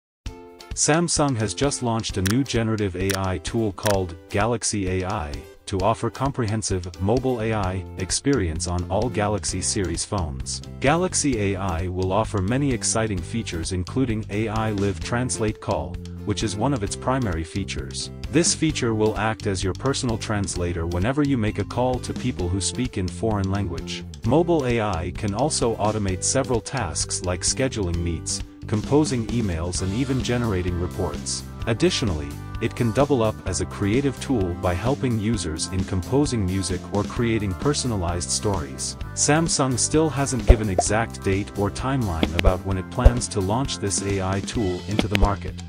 Samsung to launch Galaxy AI on all Galaxy phones
Samsung has just launched a new generative AI tool called ‘Galaxy AI’ to offer comprehensive ‘mobile AI’ experience on all Galaxy series phones.